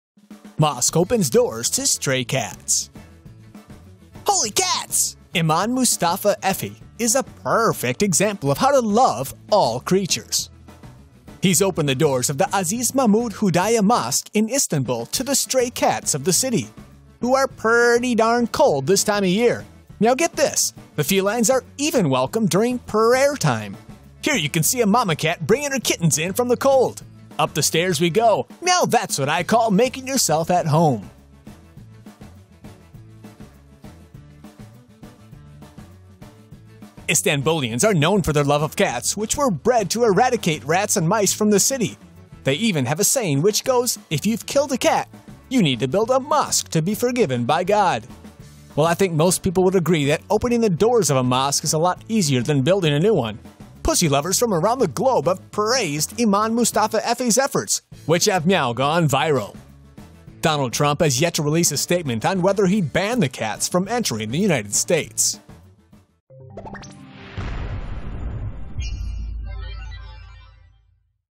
Cat videos: Imam Mustafa Efe welcomes stray cats into the Aziz Mahmud Hüdayi Mosque - TomoNews
ISTANBUL — Imam Mustafa Efe is a purrrfect example of how to love all creatures. Efe has opened the doors of the Aziz Mahmud Hüdayi Mosque in Istanbul to the stray cats of the city, who are darn cold this time of year. The felines are even welcome during prayer time.

Istanbullians are known for their love of cats which were bred to eradicate rats and mice from the city. They even have a saying which goes, "If you've killed a cat, you need to build a mosque to be forgiven by God."

Well, I think most people would agree that opening the doors of a mosque is a lot easier than building a new one. Pussy lovers from around the globe have praised Imam Mustafa Efe's efforts which have meow gone viral.

Donald Trump has yet to release a statement on whether he'd ban the cats from entering the United States.